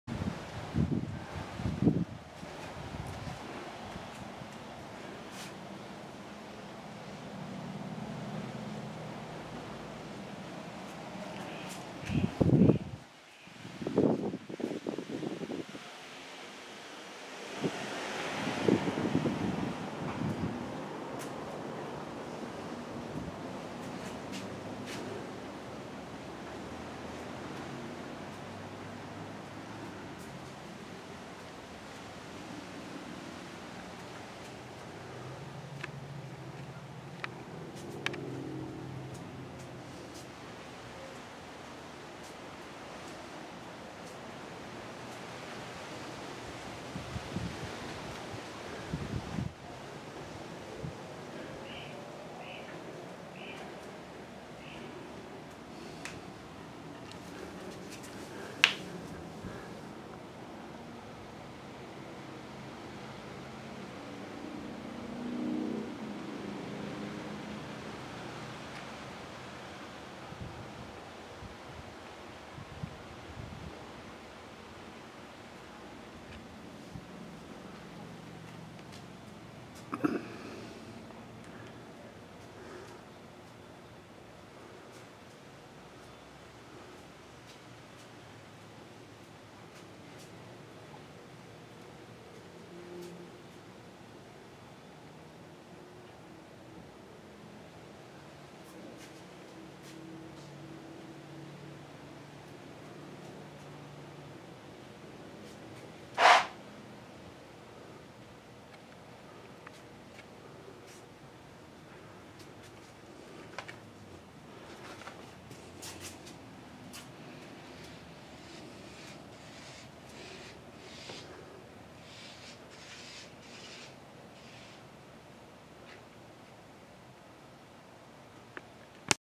1988 Cavalier Wagon Walkaround
The exterior of my 1988 Chevrolet Cavalier Station Wagon.